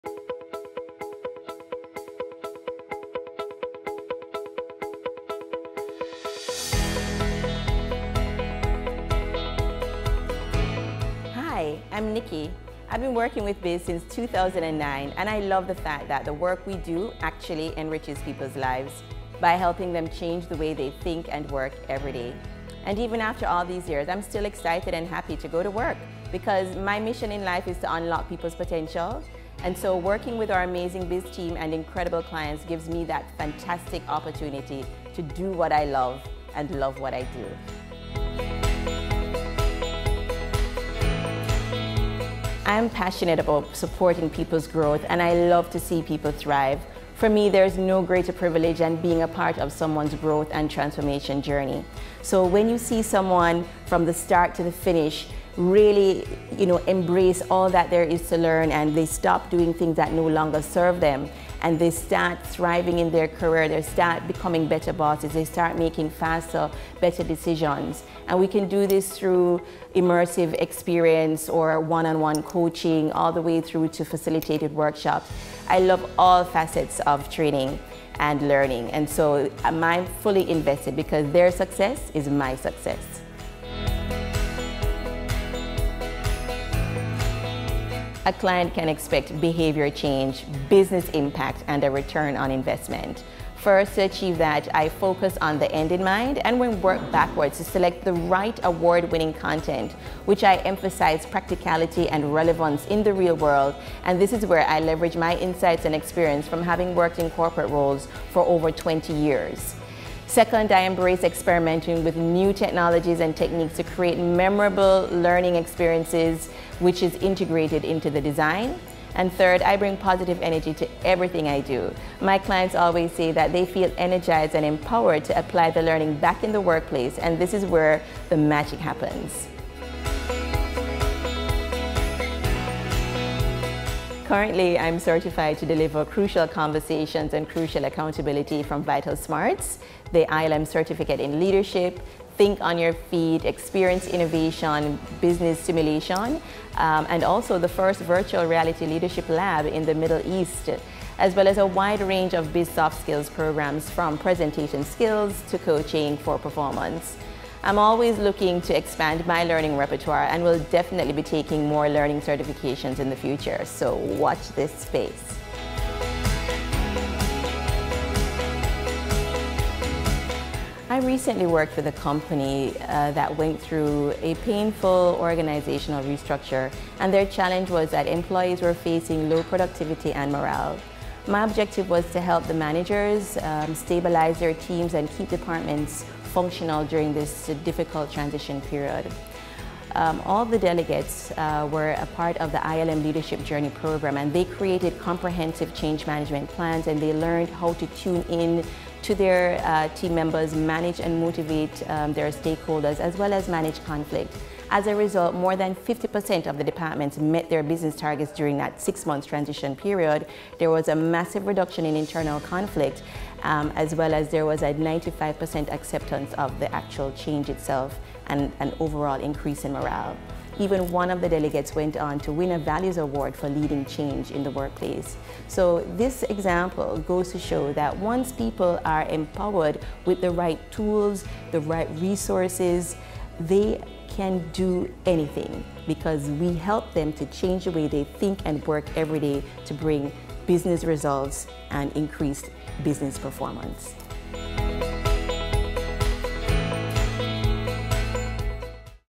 Nicky Samuels | Senior Learning Consultant | Biz Group
Since 2008, I’ve dedicated my career to support over 8,000 business professionals overcome many of the challenges of working in volatile, uncertain, complex and ambiguous work environments. Through purposeful design and delivery of experiential learning journeys sprinkled with a touch of inspiration, thousands have unlocked their potential and transformed their leadership practice, engagement and impact within numerous organisations.

ABOUT ME:
I am passionate about leadership development and enjoy the transformative process with emphasis in building effective communication skills, emotional intelligence, productivity, team engagement, personal branding, creativity and innovation. Curating the latest thinking and innovative methodologies to ensure that training needs are met to motivate me to create highly relevant and practical interventions for teams to thrive.

Prior to consultancy, I gained 12 years corporate experience and worked my way up the ladder in leading multi-national organisations with my final corporate role as General Manager for McCann-Erickson Jamaica, a fortune 500 global marketing firm, as the first female and youngest to hold this regional position.  This experience gives me unique leadership insights that allow me to connect with participants on every level.

CERTIFICATIONS:
MBA, University of New Orleans
Certified NLP & Time Line Therapy Practitioner and Coach, ABNLP
Accredited Leadership Trainer, ILM
Master Trainer - Crucial Conversations, Vital Smarts
Certified Crucial Accountability Facilitator, VitalSmarts
Certified Experience Innovation Facilitator, ExperiencePoint
Certified Facilitator, Think on Your Feet
Certified Virtual Reality Facilitator, Jenson8
Certified Virtual Instructor
INDUSTRIES:
FMCG, Retail, Pharmaceutical, Banking, Telecommunication, Healthcare, Manufacturing, Government, Hospitality, Media & Entertainment, Automotive, Logistics

WHAT CLIENTS SAY:
"You changed my life! I want to thank you for all your efforts that made a difference to my professional journey. It not only got me the opportunity to move to a regional position, but to over achieve in my new position."